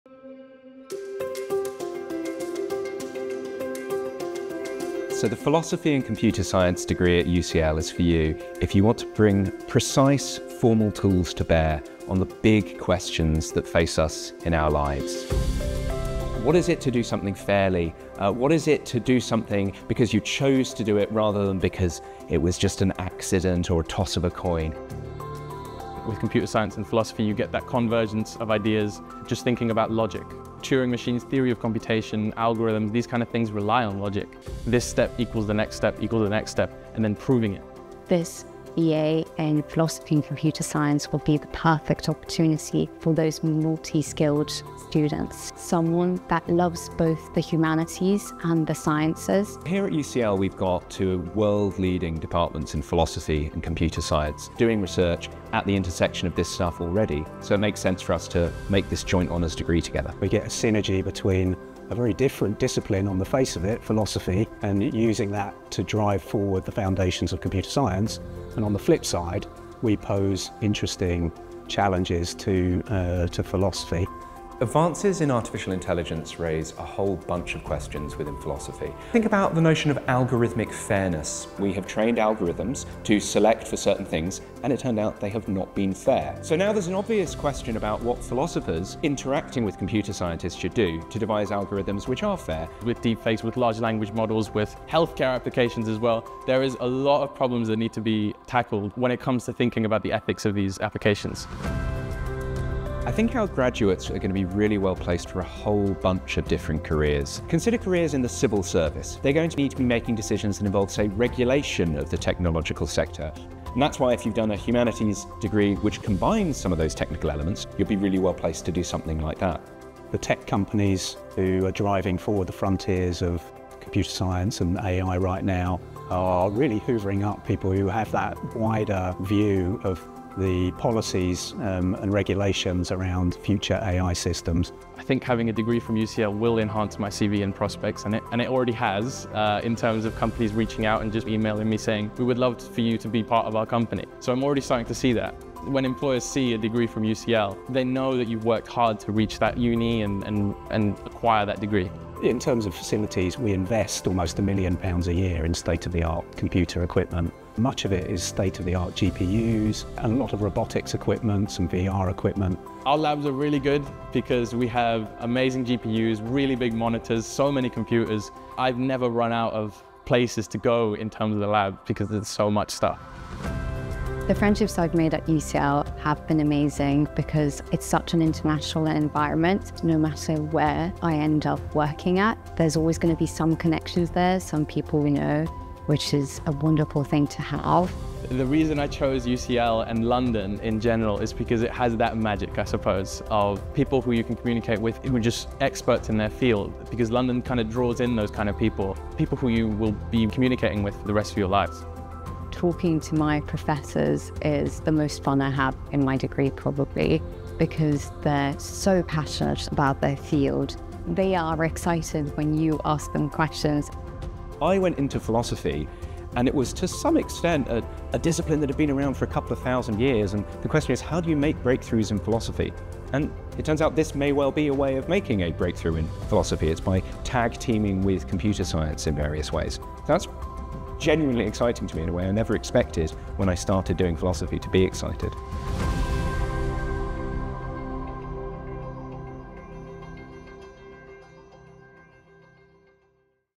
Philosophy and Computer Science BA - UCL
Humanities students must understand our increasingly digital world. Computer scientists must think philosophically. Our new undergraduate degree in Philosophy and Computer Science will provide you with the opportunity to master both.